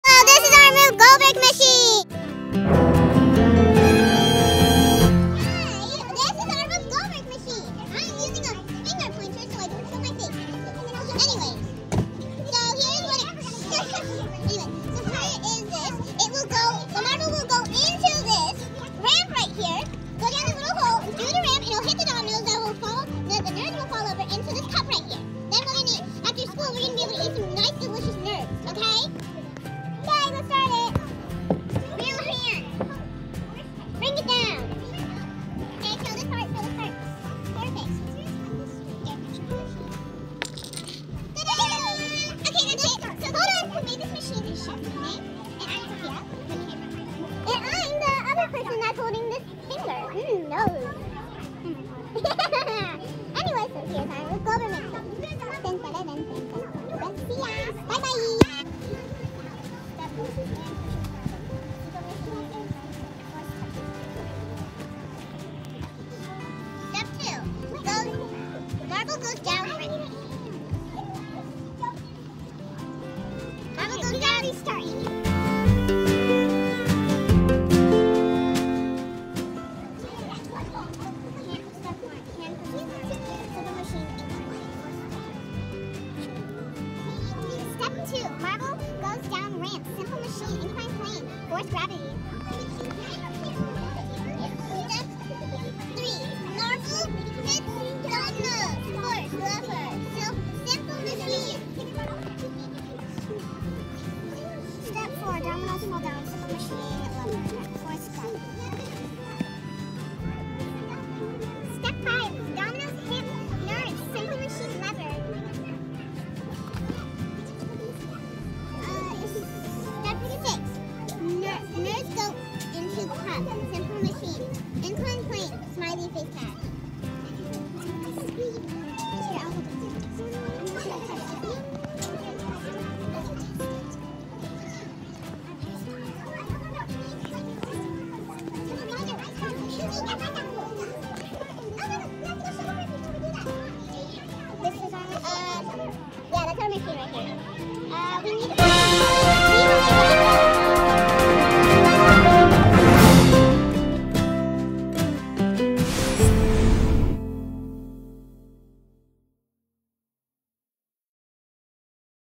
Fox Cafe ☕️ (Shake & Pour Nerds)
2023 SVUSD Rube Goldberg Challenge: Shake & Pour Nerds
Saddleback Valley Unified School District
Trabuco Elementary, Grades 2&3

0:00 Introduction
0:39 Rube Goldberg Machine in Action
1:19 Rube Goldberg Steps (Chain Reaction, Simple Machine, & Force)